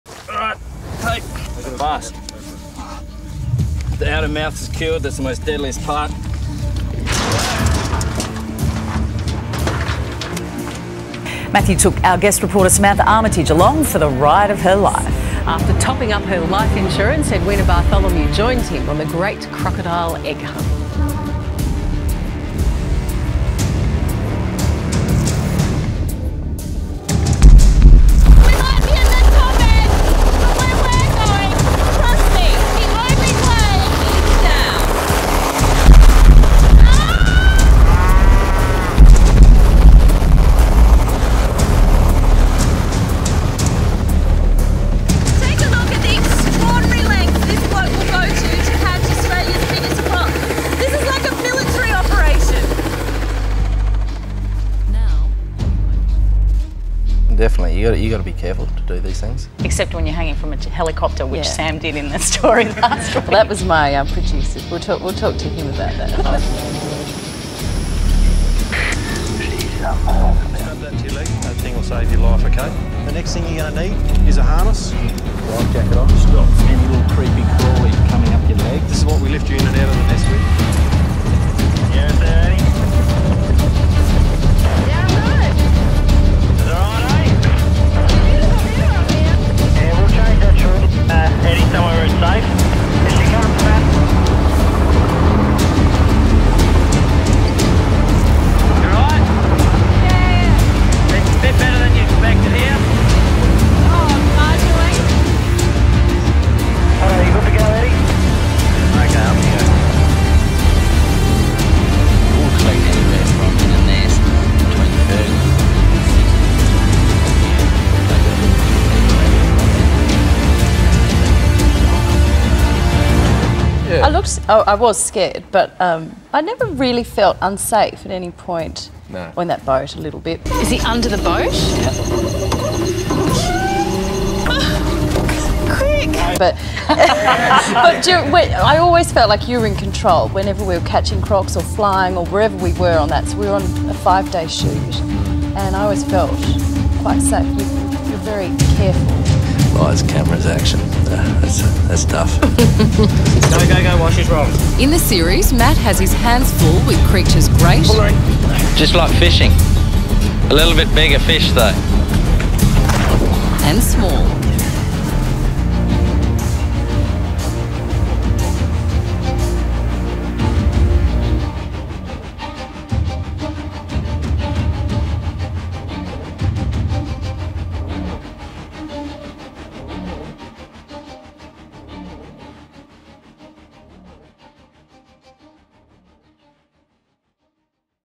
Outback Wrangler Matt Wright slings TV stars Sam and Eddie into croc nests (Watch)
Outback Wrangler Matt Wright and his colleagues, including one of the Northern Territory’s most powerful men, appear to have been flouting flight rules by slinging television stars – including Samantha Armytage and Edwina Bartholomew – beneath their choppers and onto crocodile nests in front of millions of viewers.